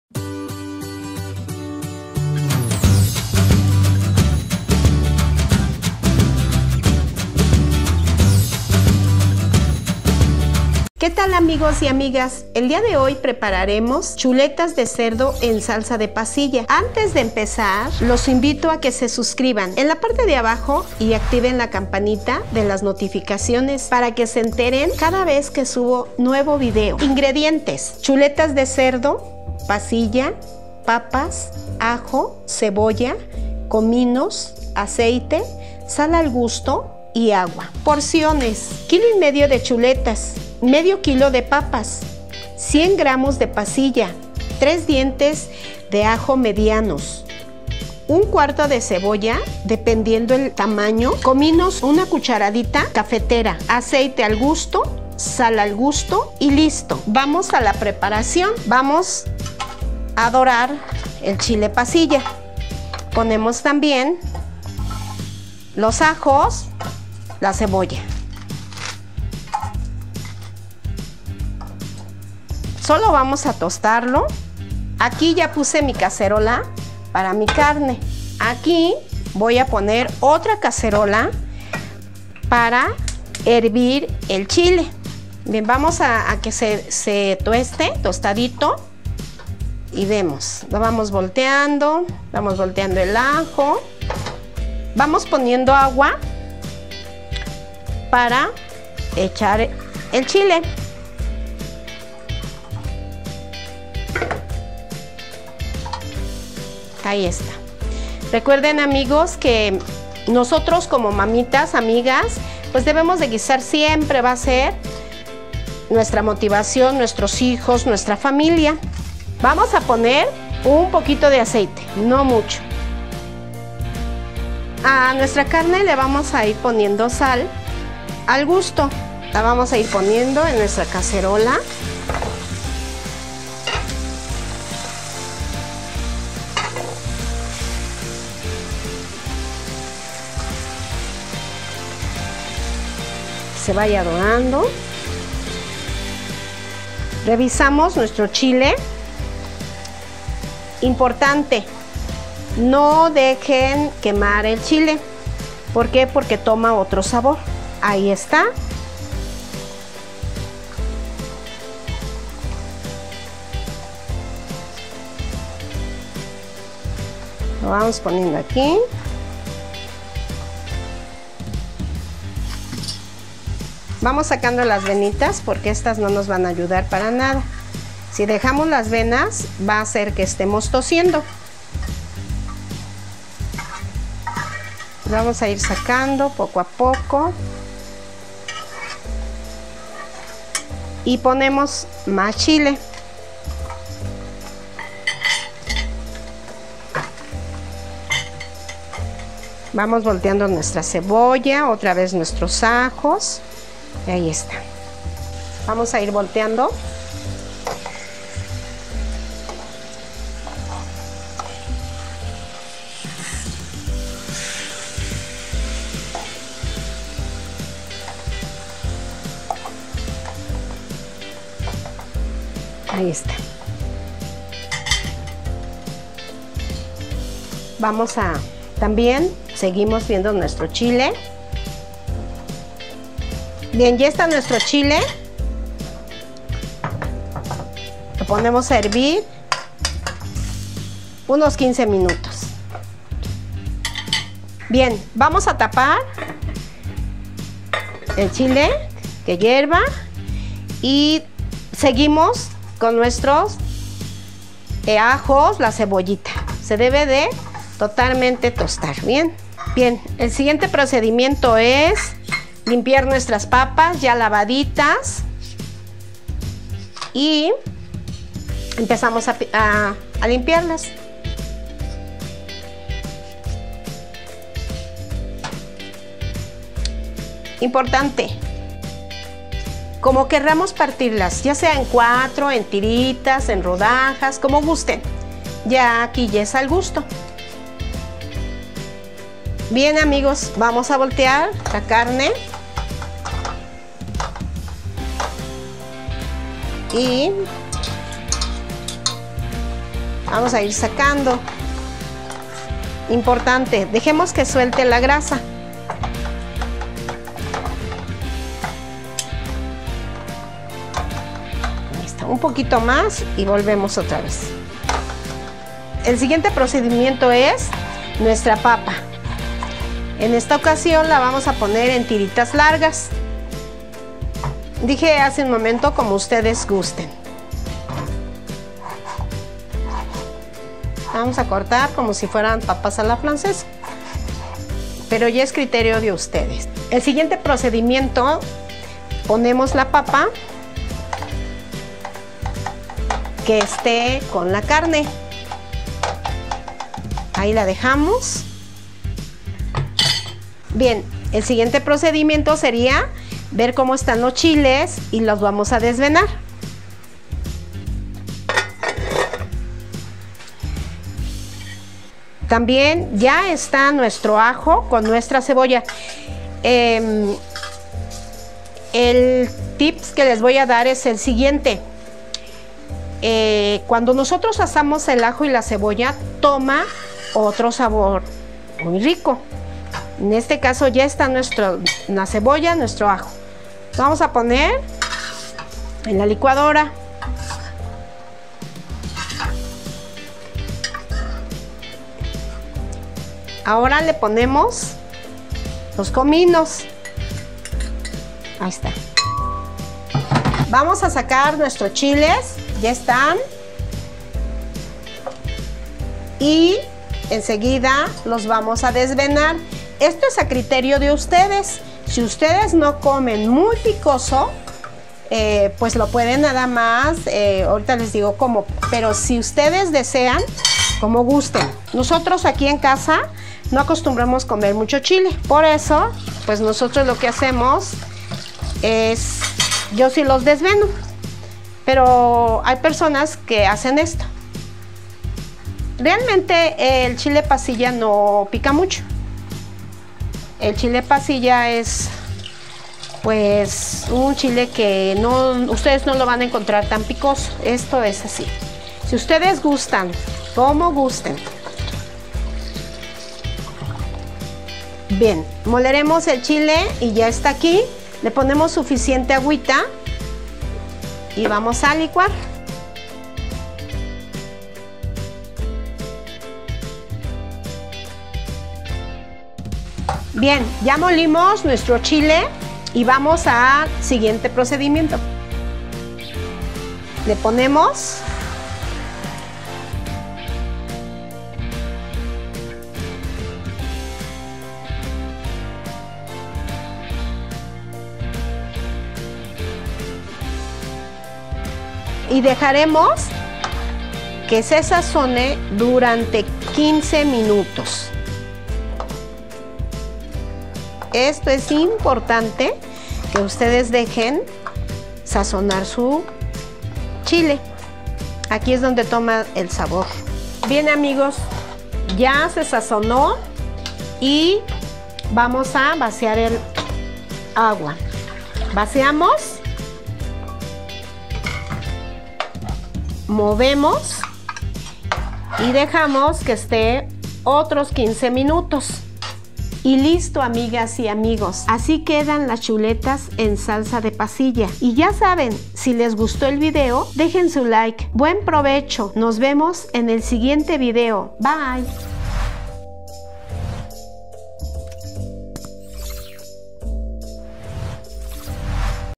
CHULETAS DE PUERCO EN CHILE PASILLA - Elia Cocinando con Amor
¡Hola amigos y amigas, el día de hoy les enseñare a preparar Chuletas de puerco en chile pasilla.